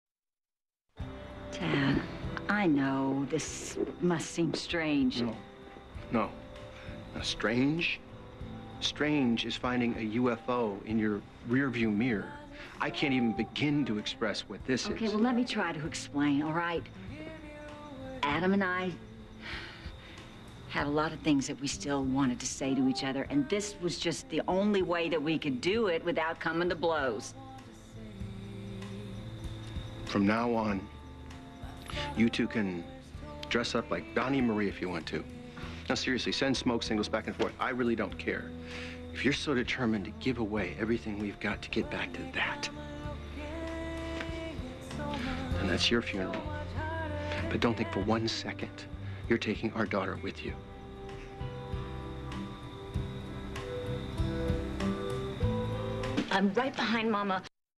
Krystal tries explaining to Tad about her and Adam's...
relationship to try and understand it. But Tad can't.